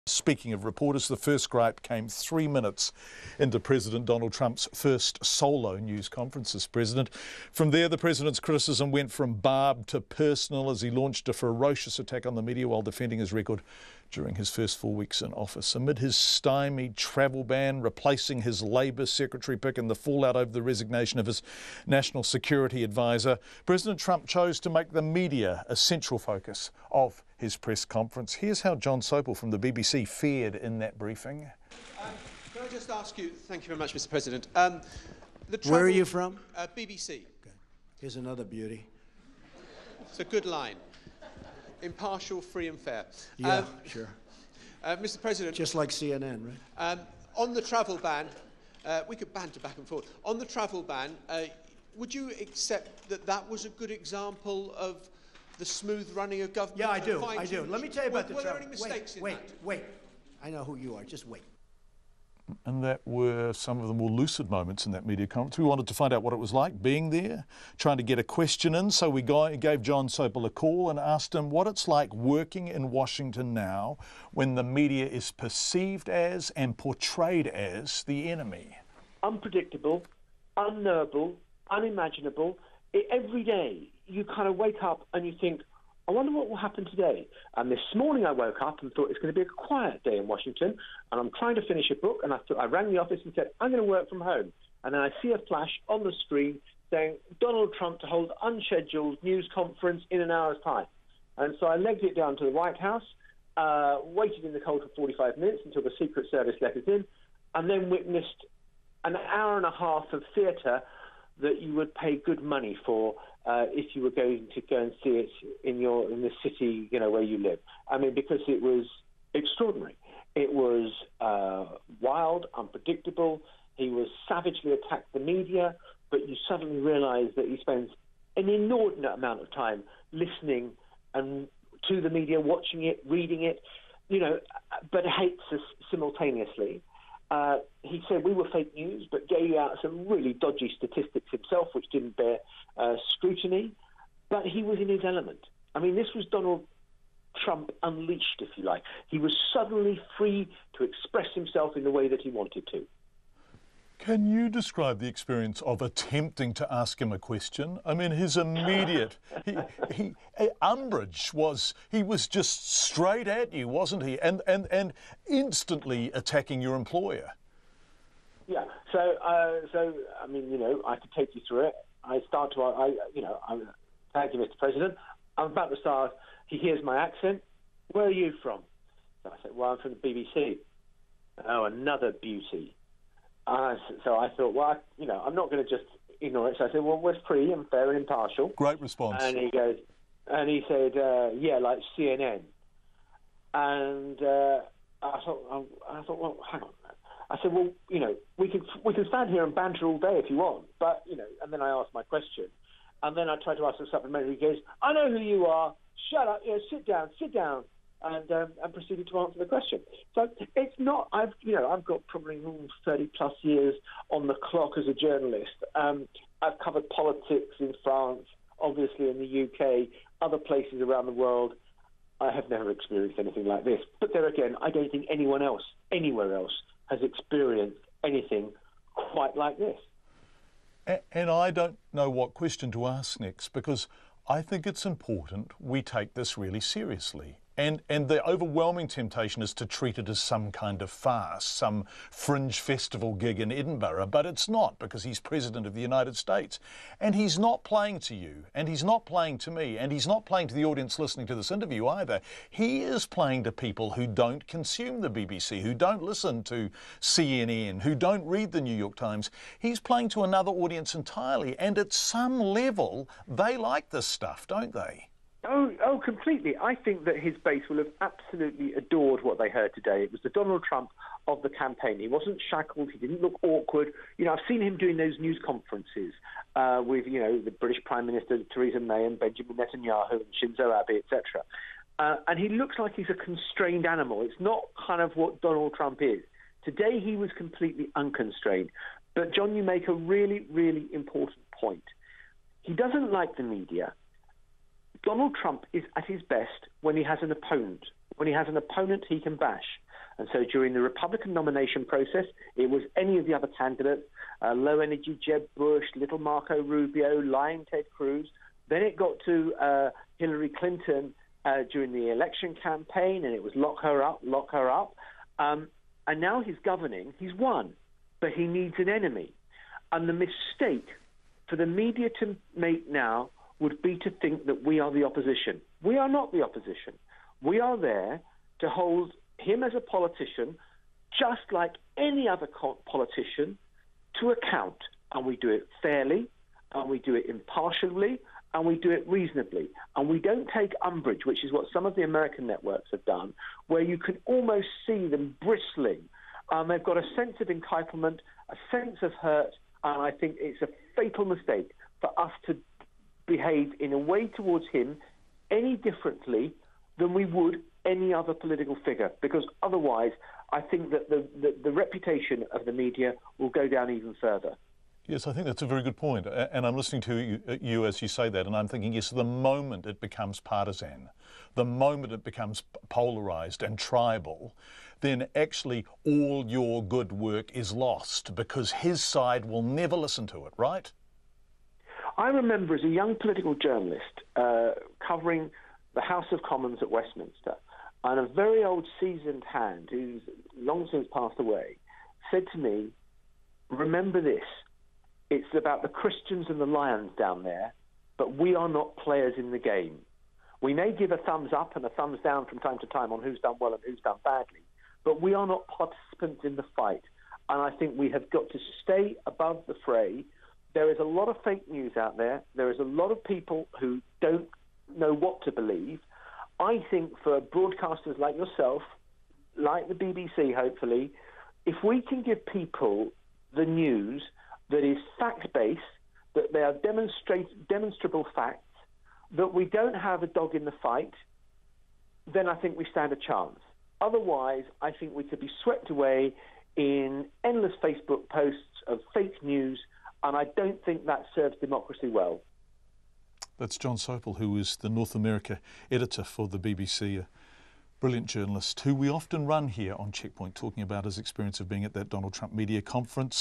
Veteran BBC reporter describes Trump's bizarre first solo news conference
Jon Sopel was personally attacked by US President Donald Trump when he tried to ask a question at a press conference